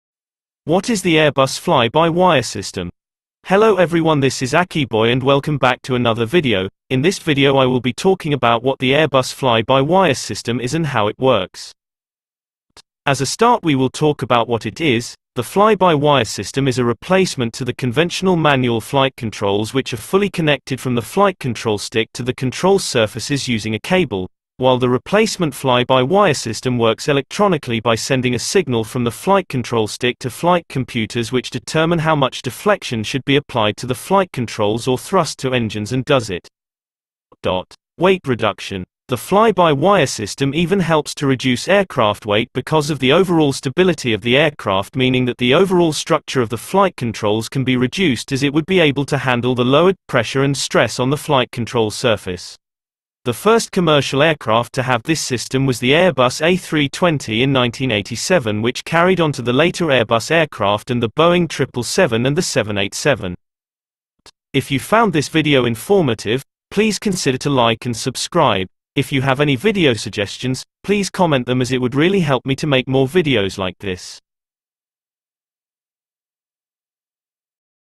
What Is The Airbus Fly By Wire System?
This Video explains what the airbus fly by wire system is and how it works.